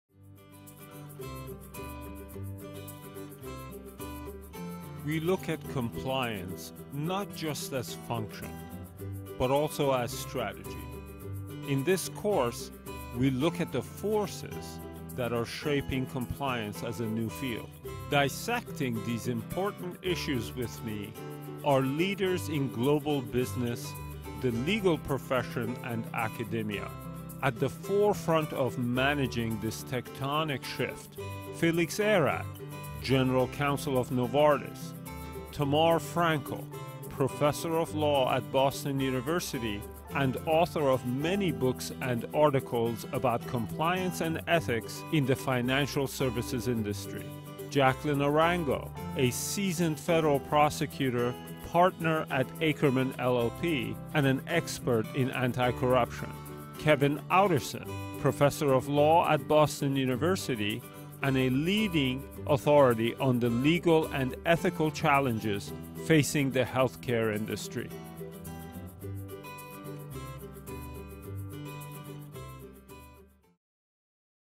Legal Risk Management Strategy for Multinational Enterprises | BUx on edX | Course About Video
Learn the core legal concepts underlying business compliance – the new paradigm in corporate accountability and risk management – in this four-part course designed for law firms seeking to expand their services for international clients and in-house counsel helping their companies go global. Hear top academics and practitioners explain the impact on transnational business operations, and the steps that enterprises can take to mitigate risk of civil and criminal liability.